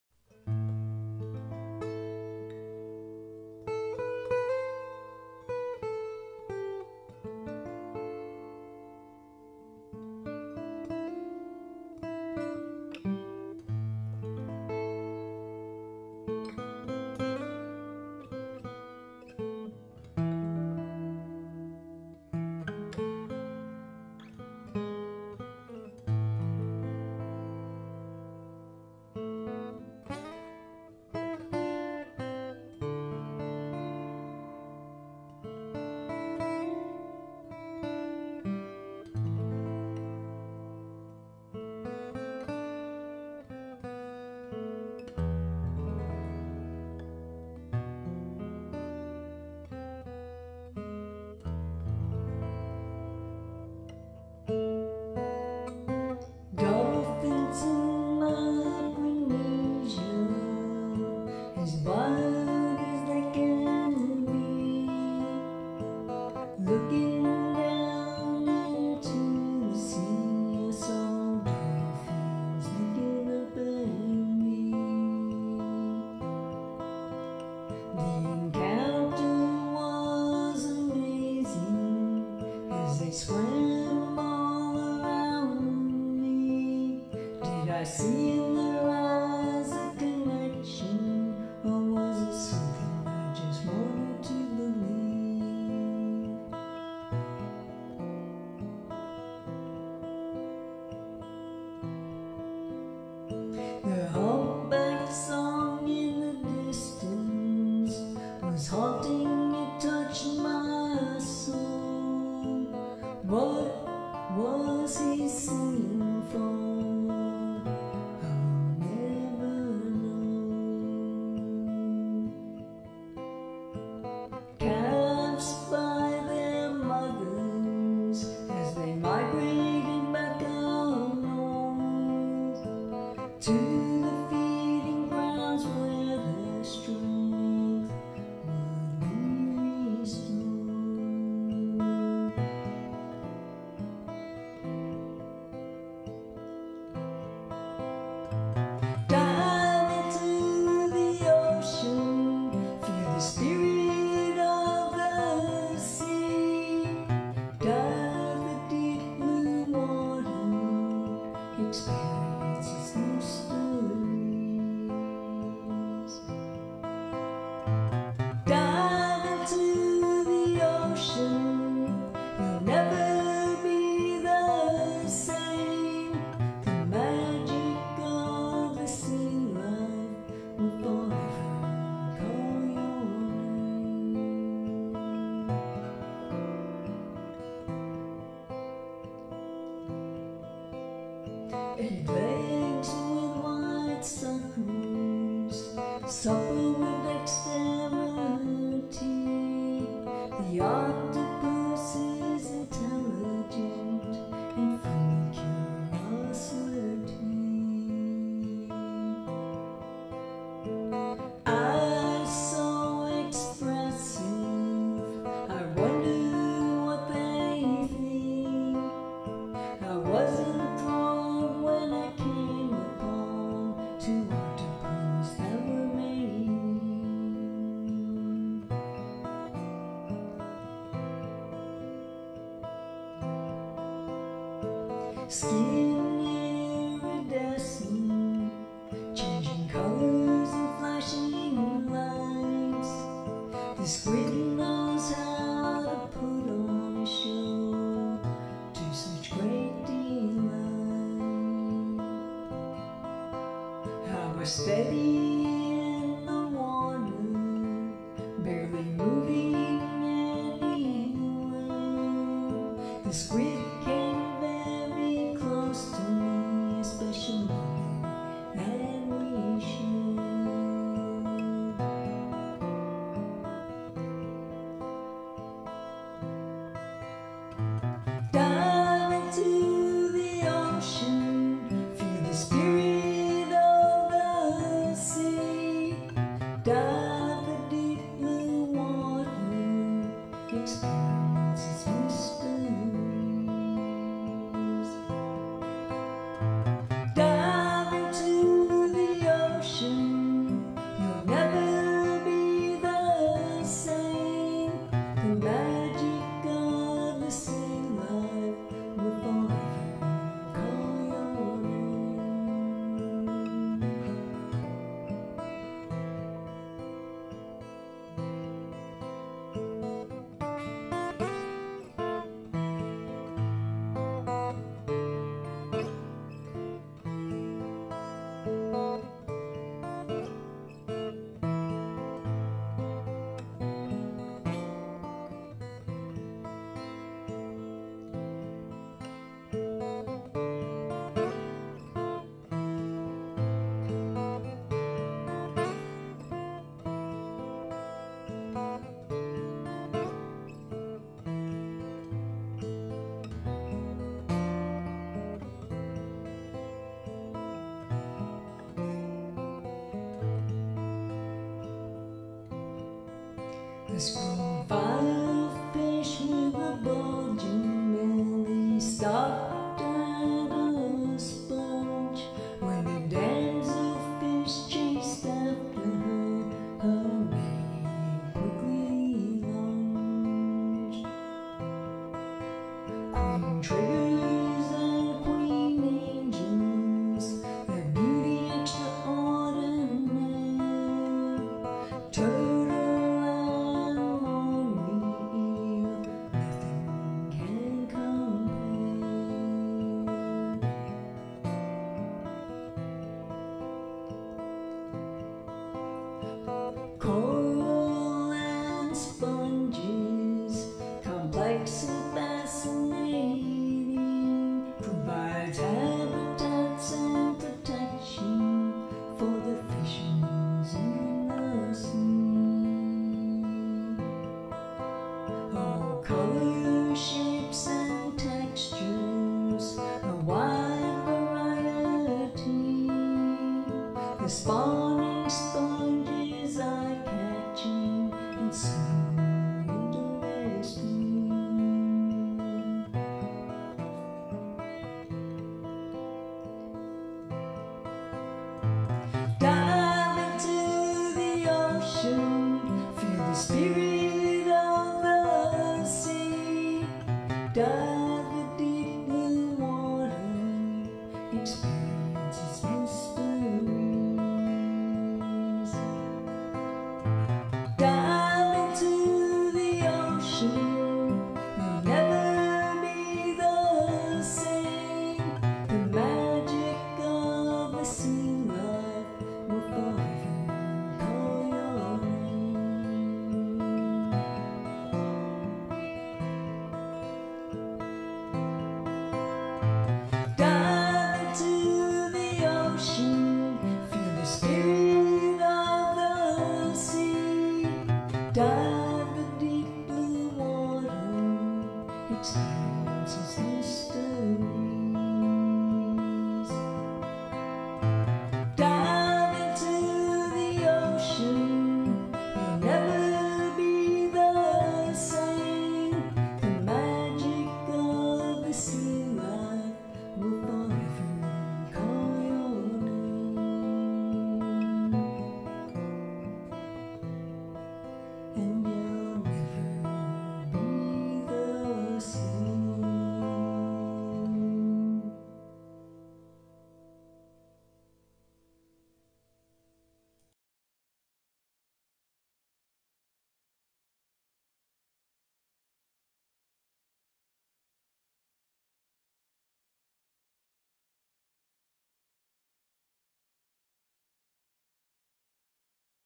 Dive into the Ocean by Linda Moroz
So many thanks and so much gratitude to Wateree Dive Center for introducing me to diving and the magical underwater world and for the great friendships in the Wateree Dive Center Family!  Thanks to all who shared the wonderful photos that are included in the video.